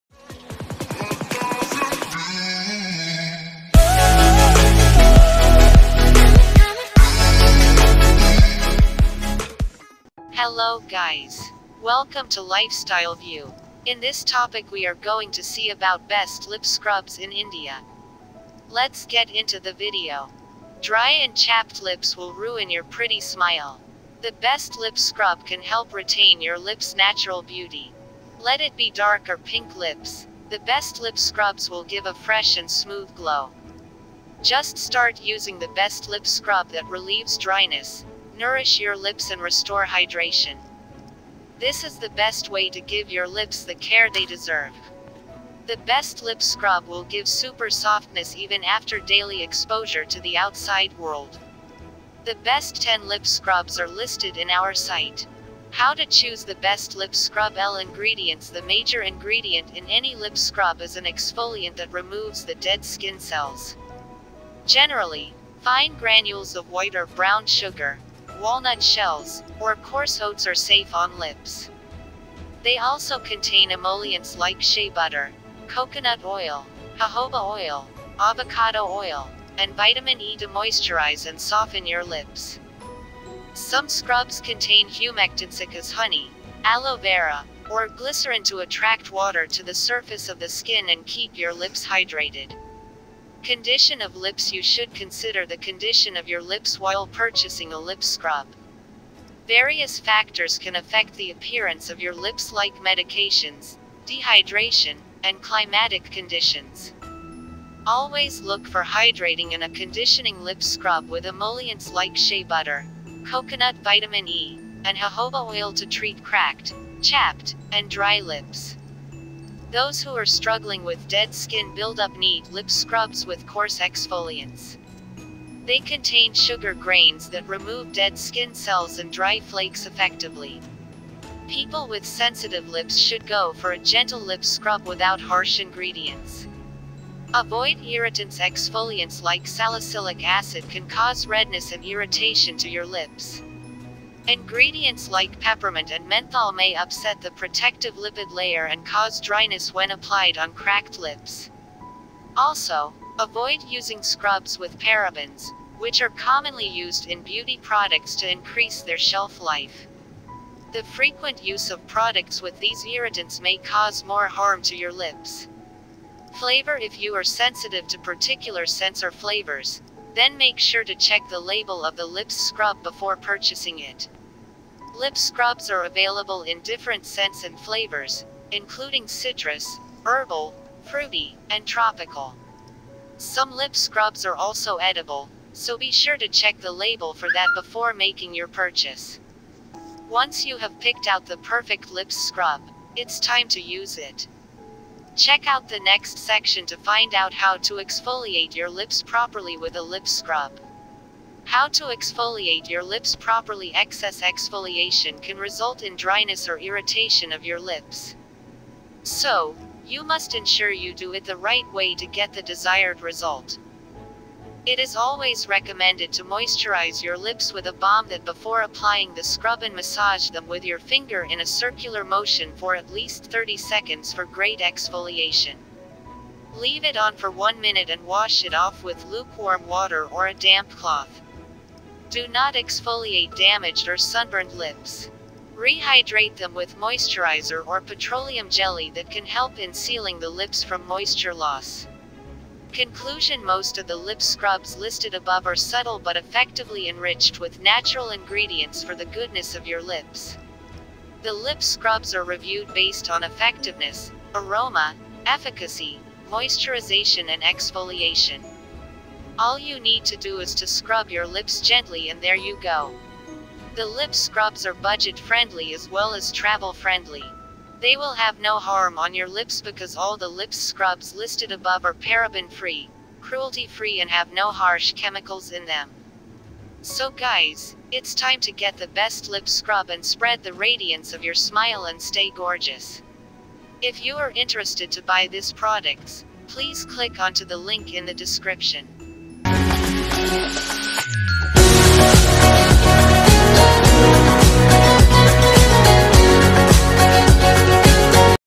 Best lipscrub in India | How to Select | Uses | Lifestyle view | Cruze Entertainments
Let's dive in & take a look at lip scrubs! Here why you need to add this lip scrubs to your skin care routine!